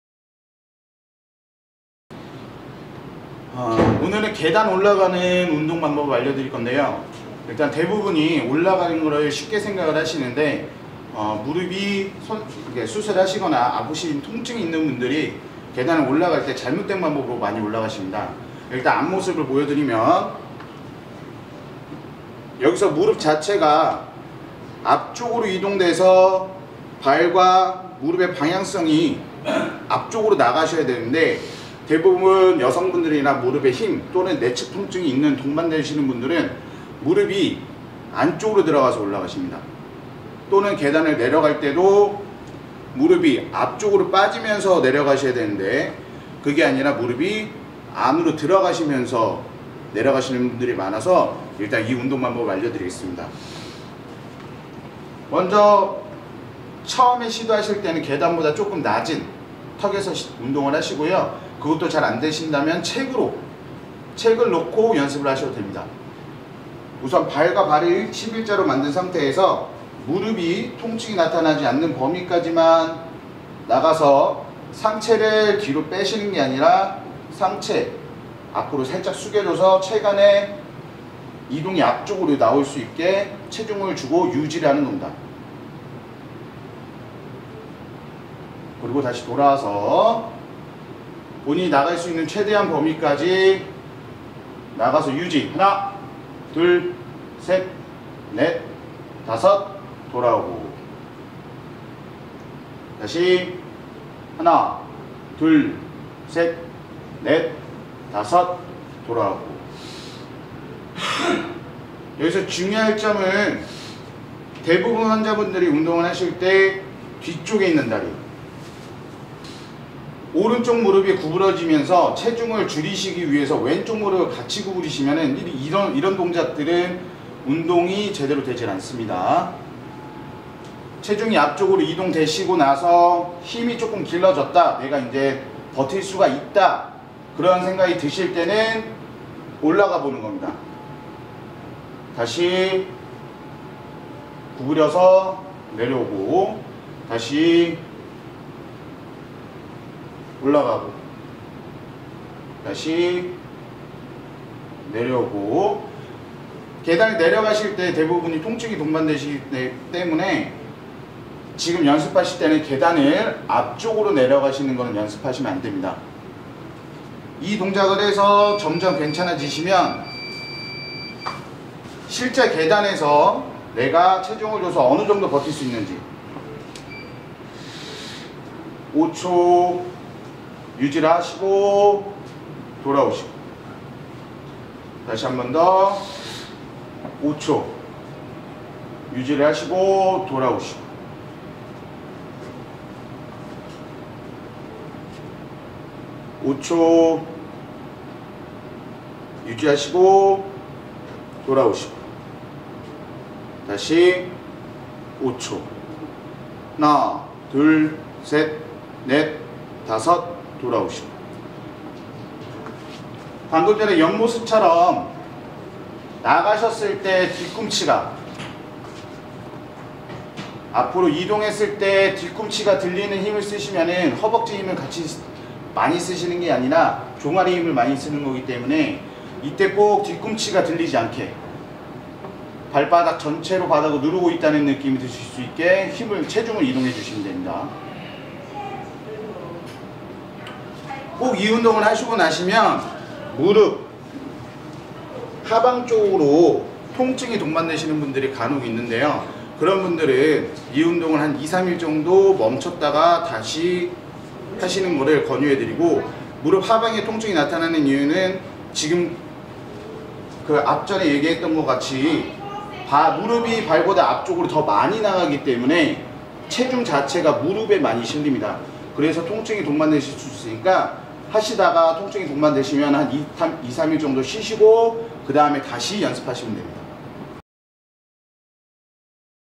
무릎 수술 후 계단 운동, 광명새움병원 김상운 도수치료사
무릎 수술 후 운동은 전문의와 충분한 상담 후에 진행하시길 바랍니다.

카카오톡 : 광명새움병원
병원주소 : 경기도 광명시 오리로 518